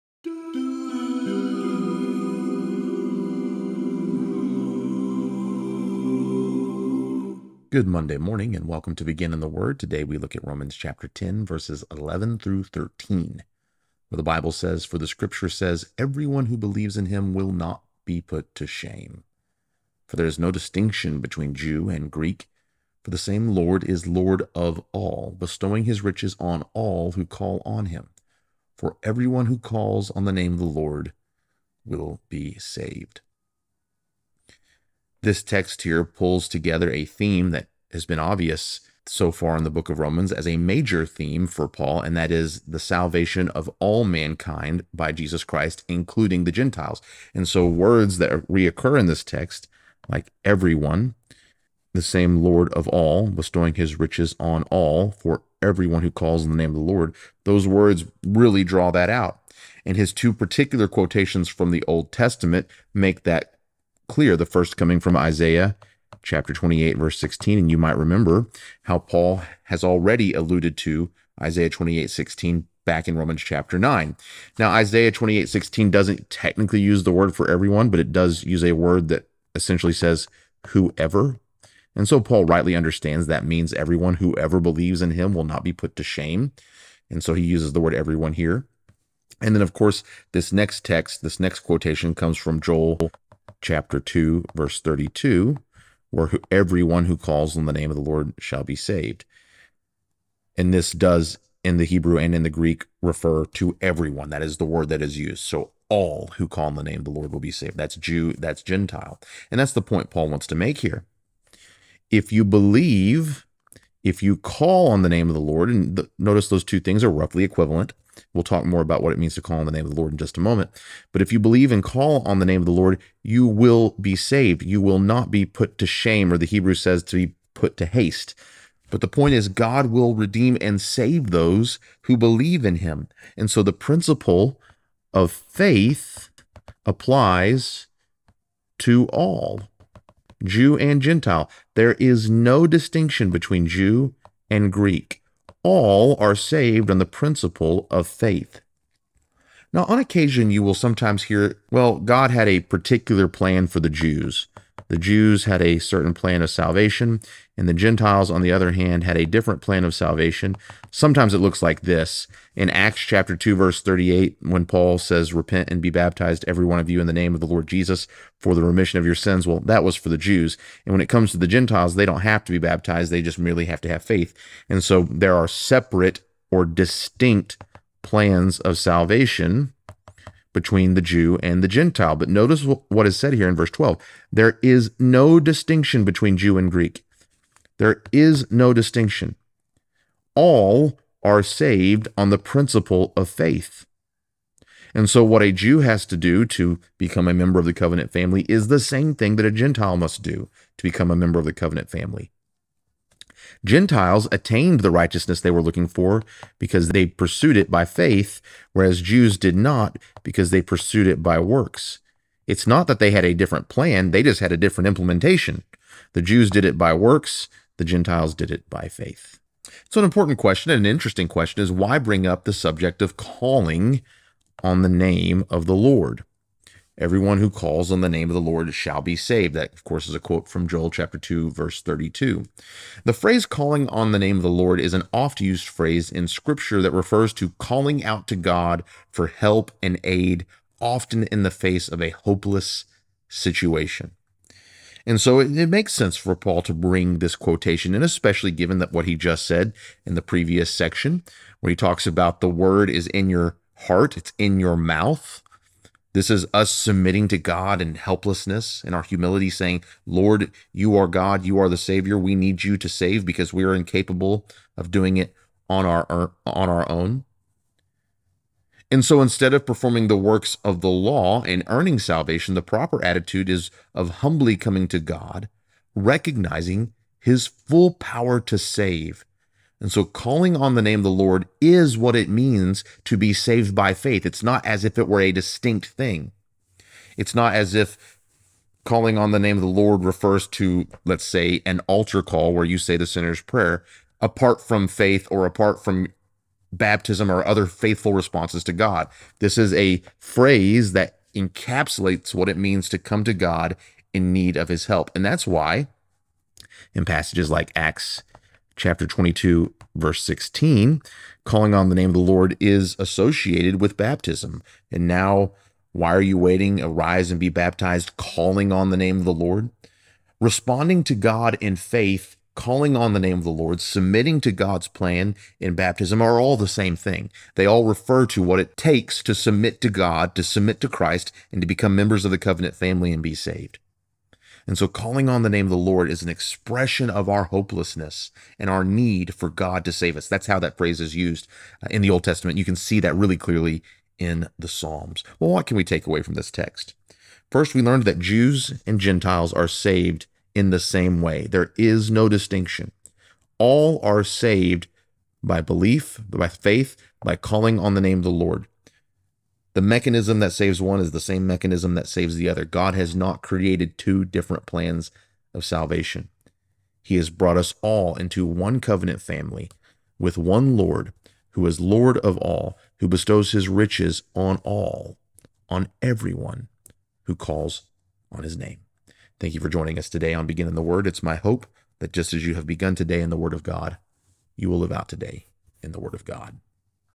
Romans 10:11-13: Everyone Who Calls on the Name of the Lord Will Be Saved
11 For the Scripture says, “Everyone who believes in him will not be put to shame.” 12 For there is no distinction between Jew and Greek; for the same Lord is Lord of all, bestowing his riches on all who call on him. 13 For “everyone who calls on the name of the Lord will be saved.”